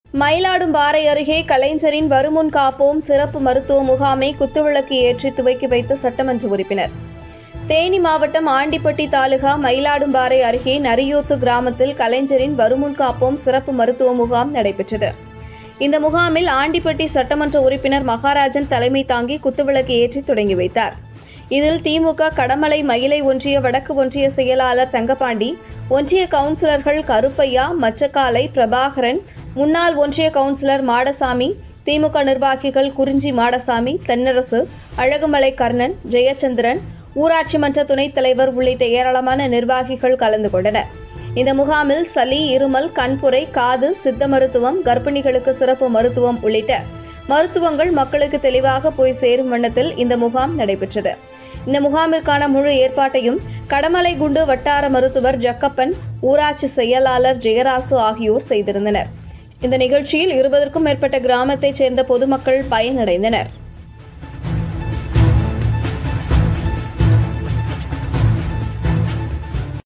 கலைஞரின் வருமுன் காப்போம் சிறப்பு மருத்துவ முகாமை துவக்கி வைத்த சட்டமன்ற உறுப்பினர்
கலைஞரின் வருமுன் காப்போம் சிறப்பு மருத்துவ
முகாமை குத்துவிளக்கு ஏற்றி துவக்கி வைத்த சட்டமன்ற உறுப்பினர்